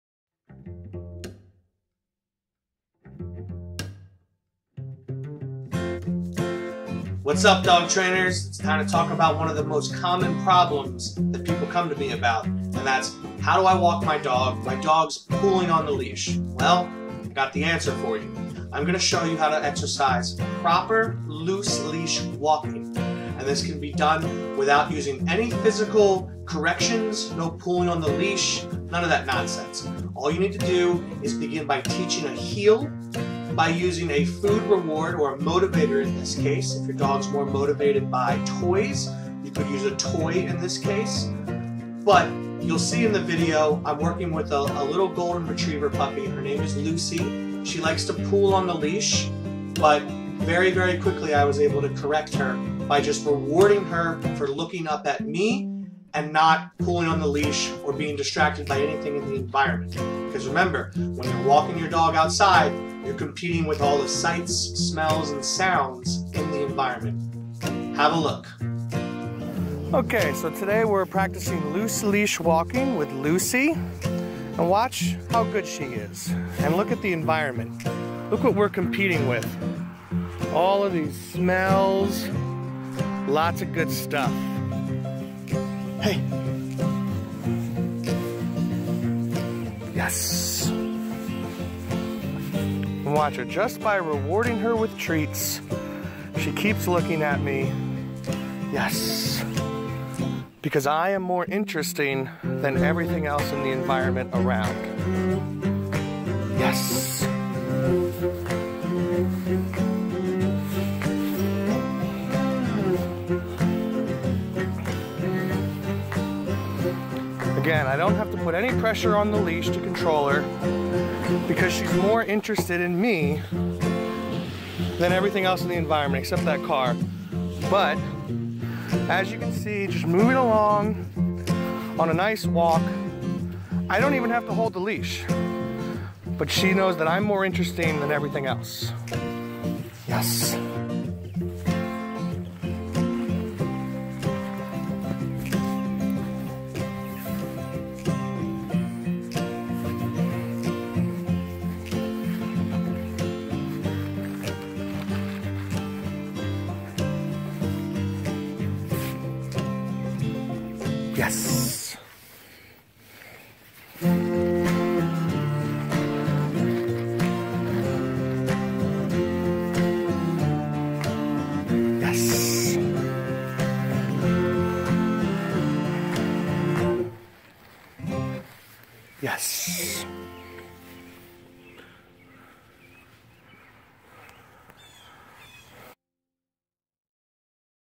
How to: Loose Leash Walking
How to walk your dog on a leash the proper way with At Attention Dog Training. This is a force-free method using a food motivator to get results.